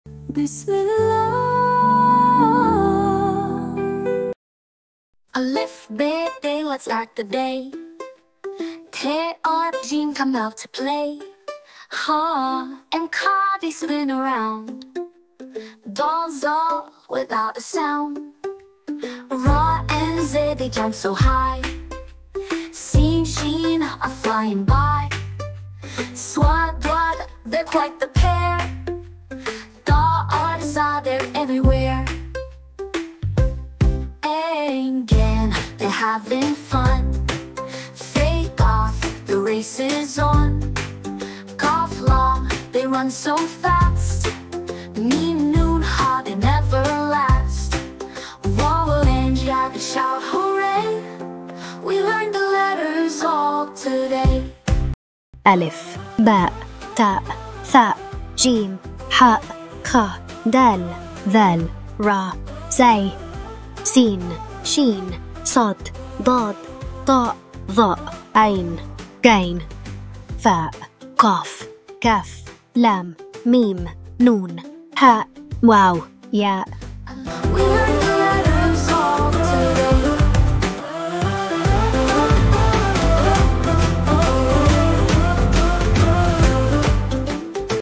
Arabic Alphabet Song for Kids | Nursery Rhymes for Kids | Islamic Kids Songs
Learn the Arabic Alphabet, Alif be te, with this fun and engaging song! Perfect for kids, beginners, and baby. Sing along to help children grasp each letter in a fun way. Great for kids who are just starting to learn Arabic!